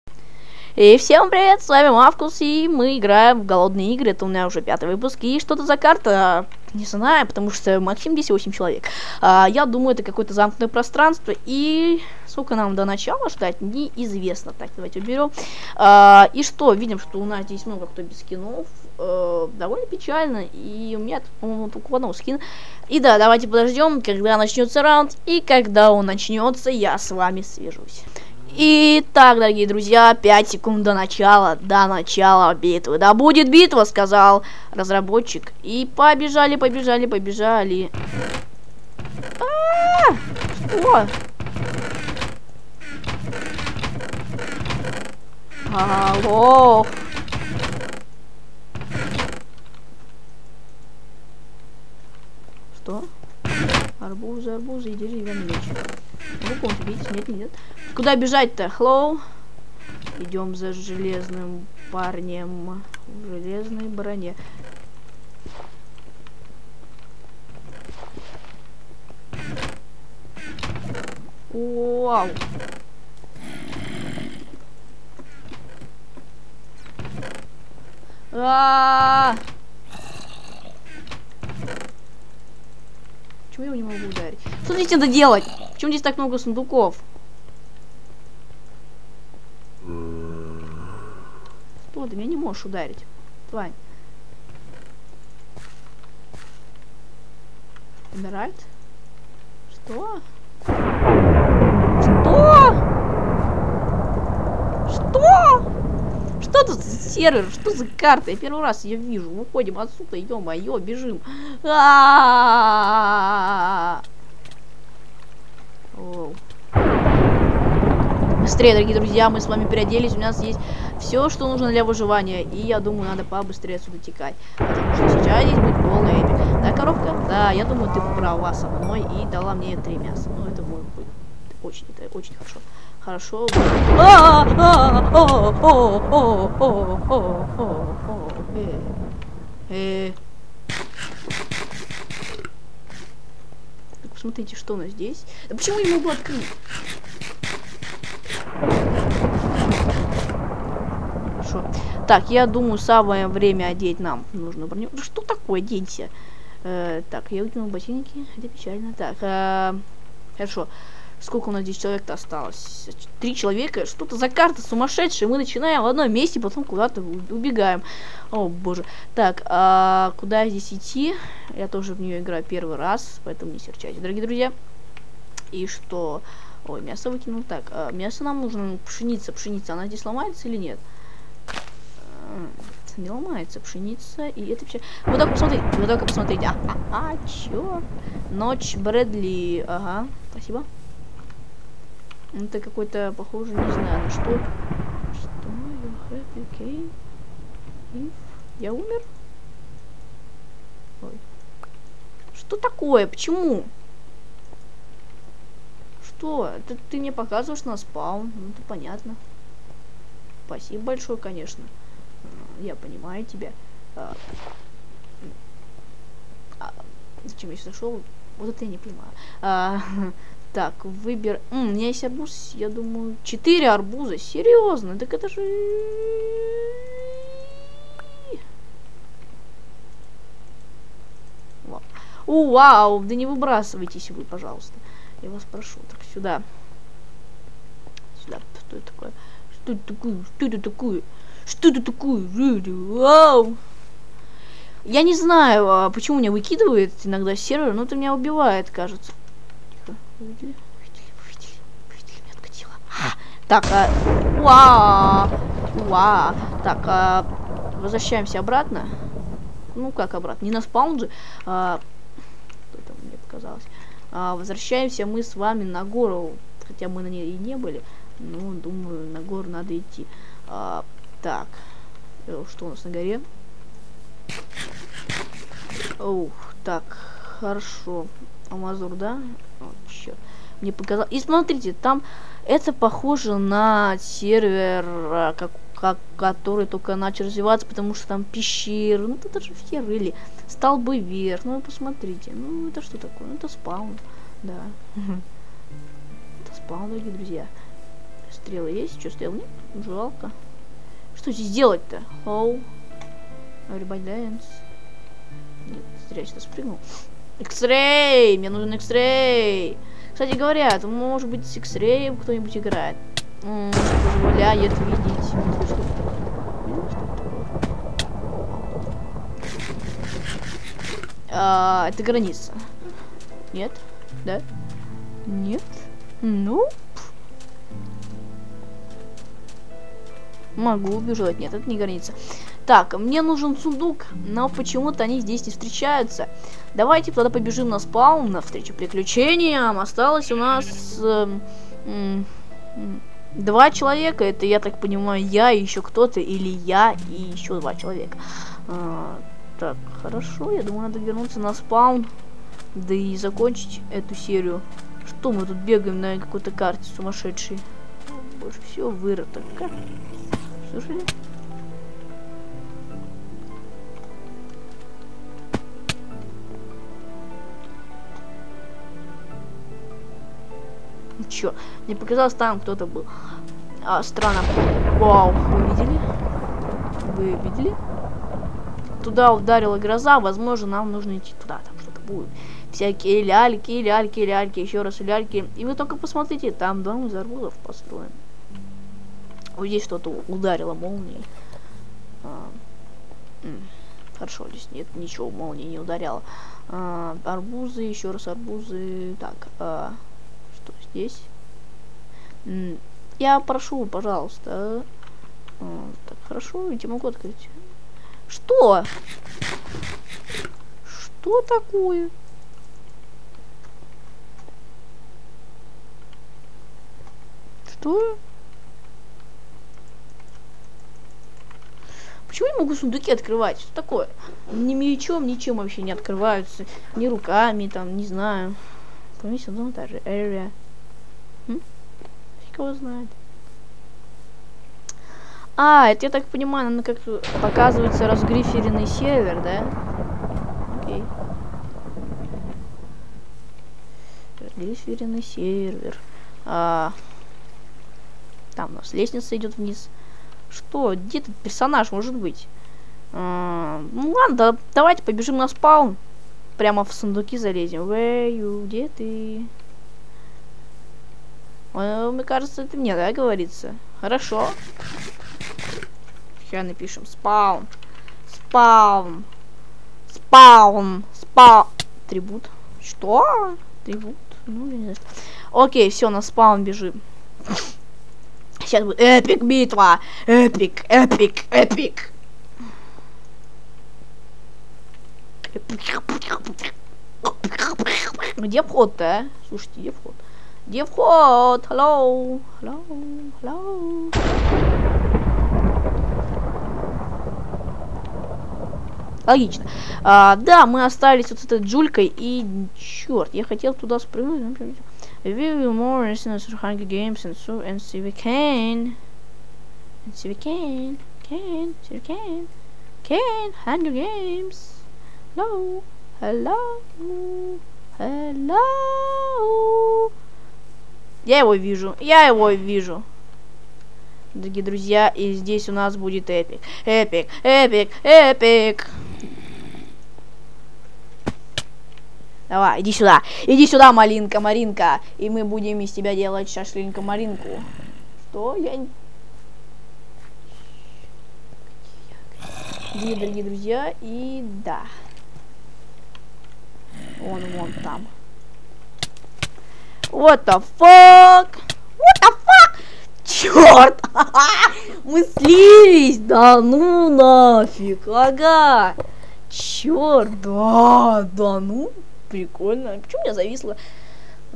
Hunger GAMES-Странная карта.
Играем в голодные игры.Смысл голодных игр в том что бы дожить до конца раунда.Если выживаешь то выигрываешь!